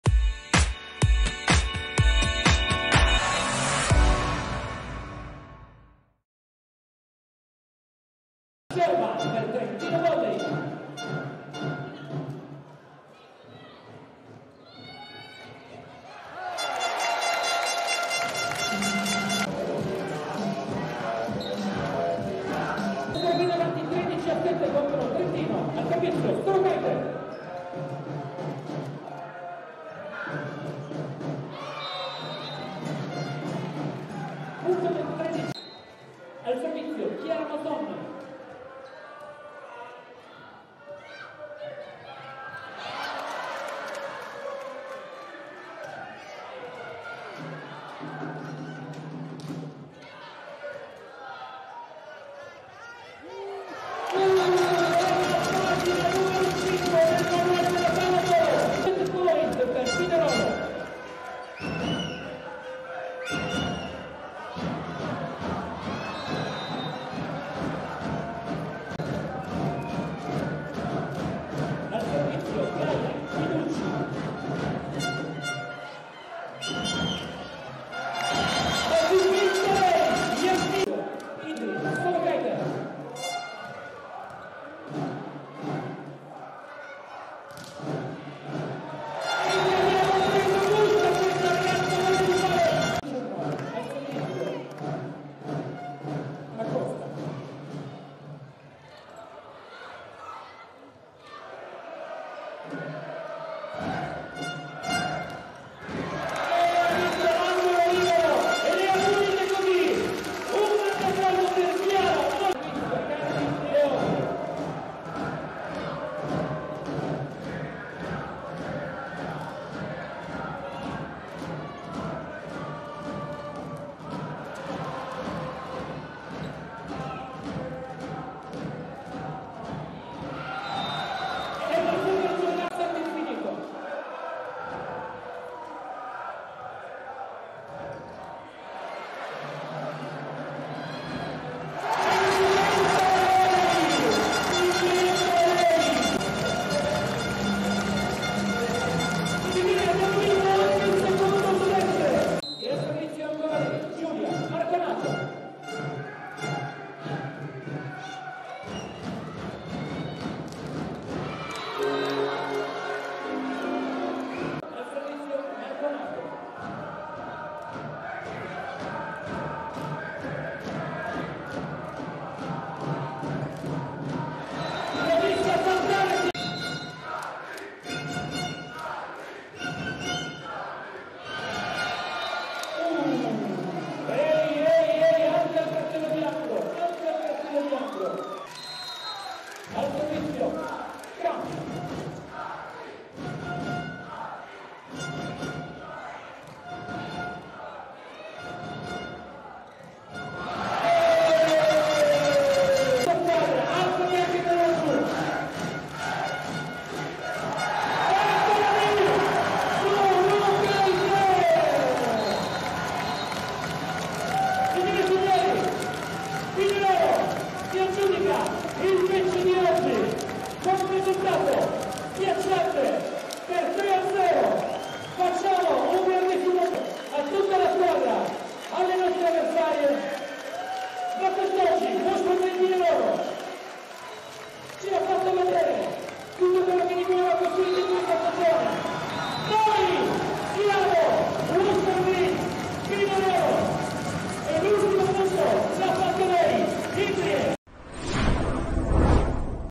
💥🏐 HIGHLIGHTS | Wash4Green Pinerolo-Itas Trentino 3-0 (26° turno di A1 femminile)
Le immagini più significative dell'incontro giocato alla a Villafranca Piemonte il 24 marzo 2024